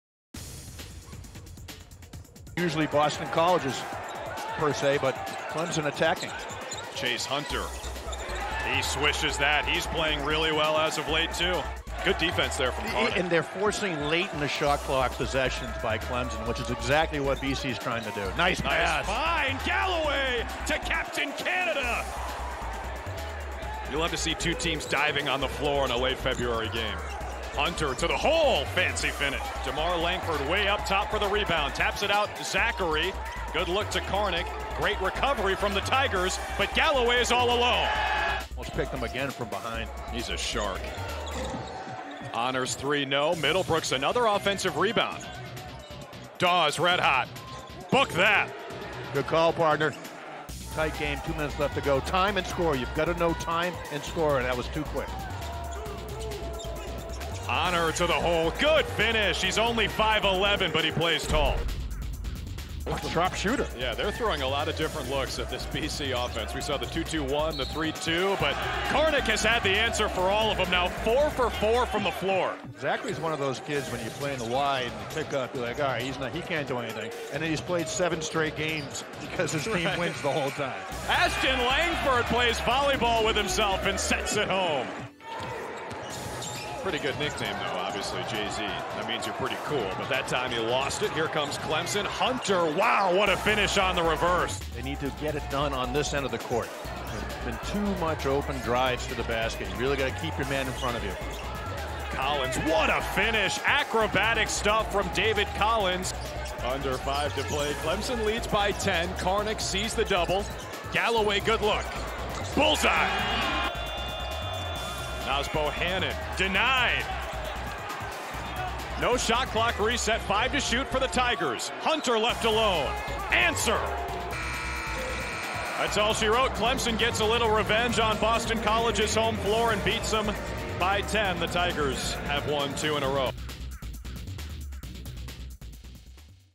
Clemson vs. Boston College Men's Basketball Highlights (2021-22)
Clemson vs. Boston College: The Clemson Tigers got a big win on the road at Boston College, 70-60. Chase Hunter topped his career-high in scoring with 23 points on 7-of-9 shooting, including 1-of-2 from three-point range while grabbing 7 rebounds and connecting on 8-of-10 foul shots to help the Tigers put the game away late. Al-Amir Dawes scored 16 points on 4-of-8 shooting from three-point range in the win. Seniors James Karnik, Makai Ashton-Langford, and Brevin Galloway fought hard for the Eagles after being honored on Senior Day, scoring 17 points, 10 points, and 11 points, respectively in the loss.
Thumbnail courtesy of: @ClemsonMBB.